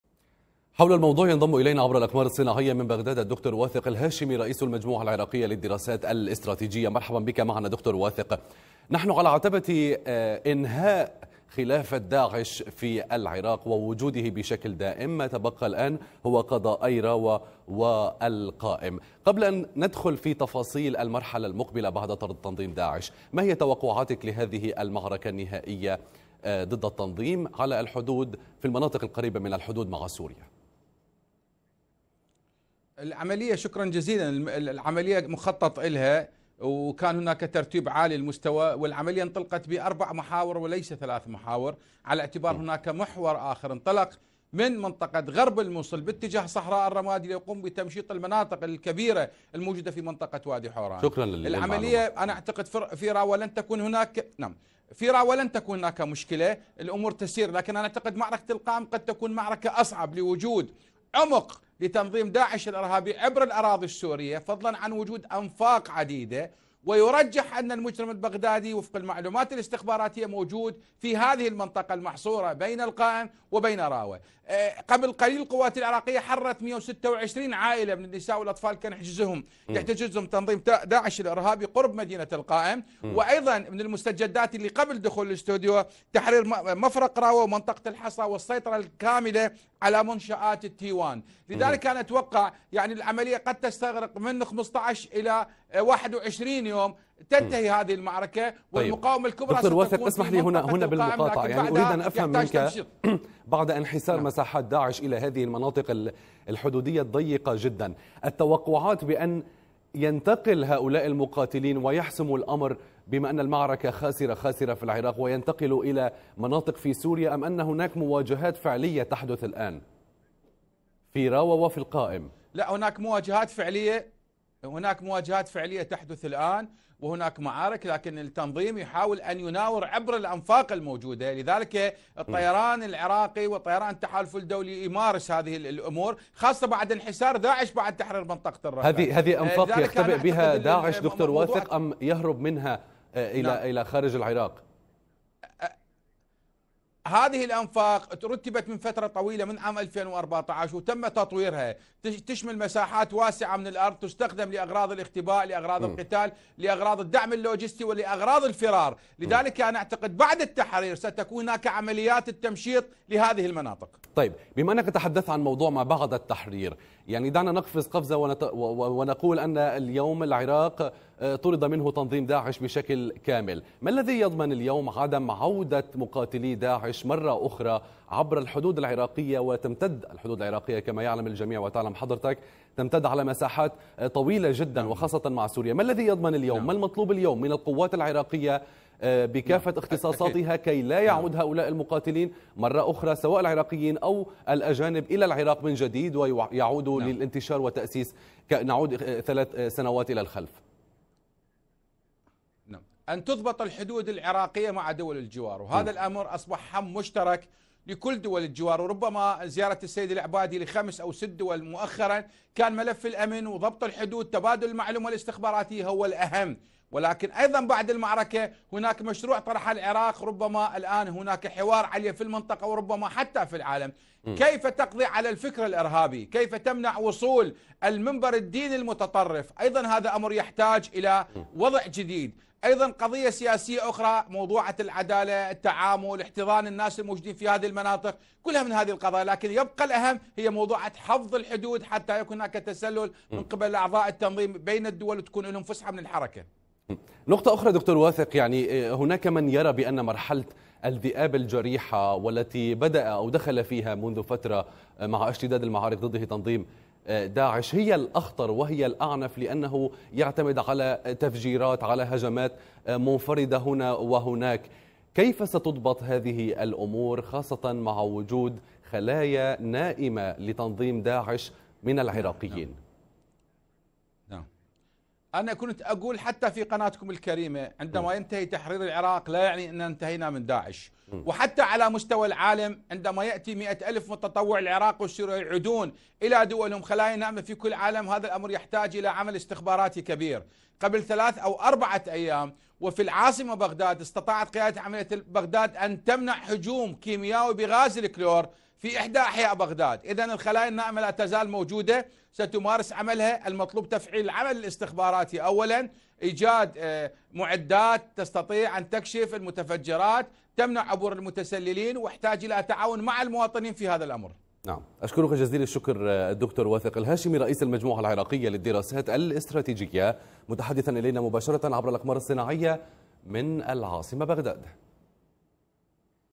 القوات العراقية تباشر عملية تحرير الشريط الحدودي مع سوريا (تفاصيل)
شاهد آيضا:
قيادي بعثي يفضح تاريخ آل الأسد!؟
بعد يومين من الضربات الجوية المركزة للجيش العراقي و التحالف_الدولي.. إنطلقت فجر الخميس عمليات تحرير الشريط الحدودي مع الجانب السوري،, آخر المساحات التي يحتلها داعش في العراق.

عملية تحرير قضاء القائم و راوه إنطلقت من ثلاثةِ محاور، الأول من مفرق عنه باتجاه راوه، والثاني من منطقة t1 تي ون باتجاه مجمع الفوسفات السكني.. أما المحور الثالث كان
من منطقة عكاشات باتجاه القضاء.

تشكيلات عدة من القوات الأمنية العراقية تشارك في هذه العمليات، منها قيادة عمليات الجزيرة.. والفرقتين السابعة والآلية الثامنة فضلا عن شرطة الانبار.

تعد منطقة صحراء الانبار التي انطلقت عمليات تحريرها أحدى الأوكار المهمة لمقاتلي داعش، كونها منطقة شاسعة ممتده بين محافظات نينوي وصلاح الدين والأنبار.  حيث كان ينشط داعش قبل دخوله الموصل..  ومع تحرير هذه المناطق سيطوي العراقيون صفحة تنظيم داعش السوداء.